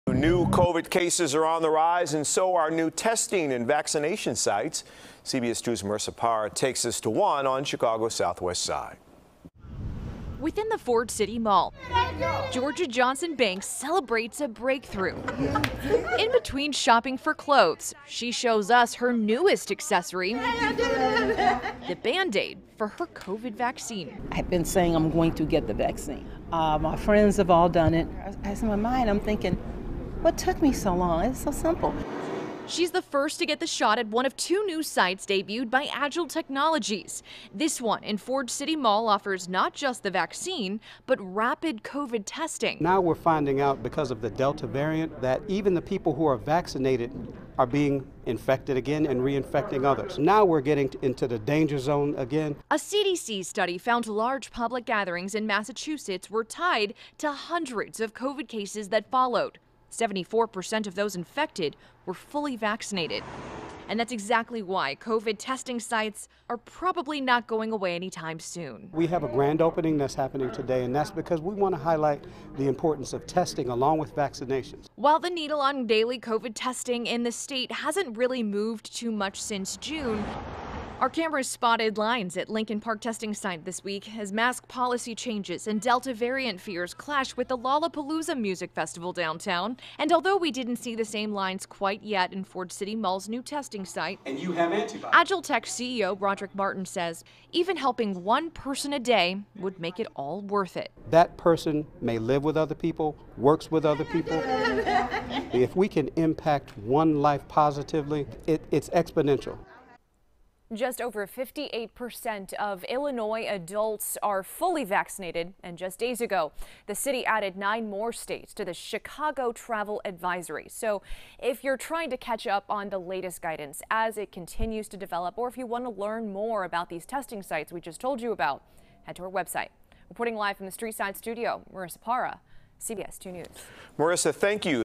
COVID-19 Testing Returns Alongside Vaccination At 2 New Chicago Area Sites
Two sites that offer free COVID testing and vaccinations opened in the Chicago area on Saturday – in yet another symptom of the Delta variant. CBS 2's Marissa Parra reports.